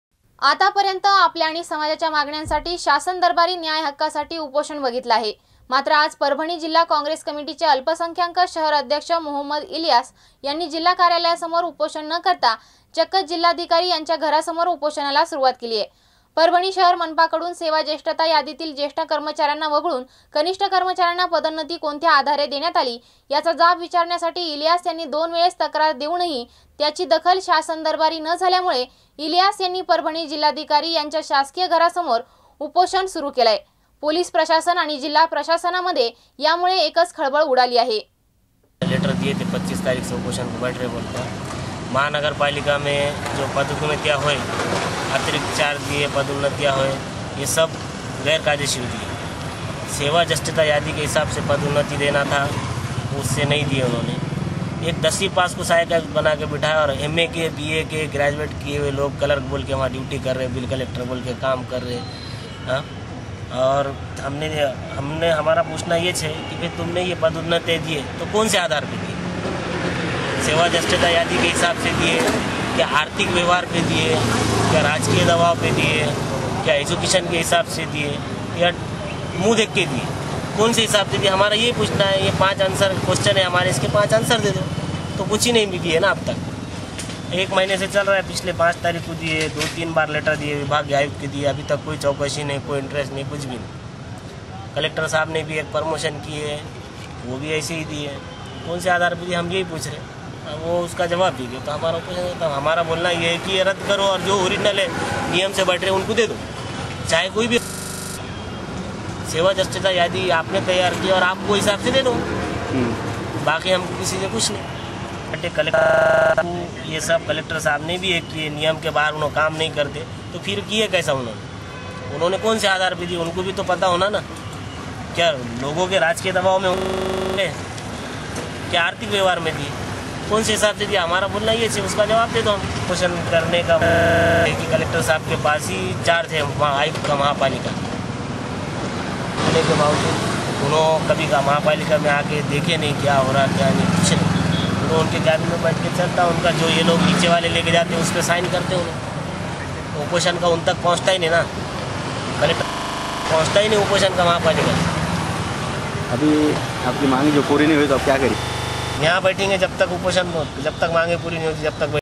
* १जिल्हाअधीकारीच्या घरासमोरच उपोषण जिल्यात सर्वत्र एकच खळबळ
आतापर्यंत आपल्या आणि समाजाच्या मागण्यासाठी शासन दरबारी न्यायहक्कासाठी उपोषण बघितले आहे. मात्र आज परभणी जिल्हा काँग्रेस कमिटीचे अल्पसंख्यांक शहर अध्यक्ष मोहंमद इलियास यांनी जिल्हा कार्यालयासमोर उपोषण न करता चक्क जिल्हाधिकारी यांच्या घरासमोर उपोषणास सुरुवात केली आहे . परभणी शहर मनपाकडून सेवा जेष्ठता यादीतील जेष्ठ कर्मचाऱ्यांना वगळून कनिष्ठ कर्मचाऱ्यांना पदोन्नती कोणत्या आधारे देण्यात आली, याचा जाब विचारण्यासाठी इलियास यांनी दोन वेळेस तक्रार देऊनही त्याची दाखल शासन दरबारी न झाल्यामुळे इलियास यांनी परभणी जिल्हाधिकारी यांच्या शासकीय घरासमोर उपोषणास बसले असून त्यामुळे पोलीस प्रशासन आणि जिल्हा प्रशासनमध्ये एकच खळबळ उडाली आहे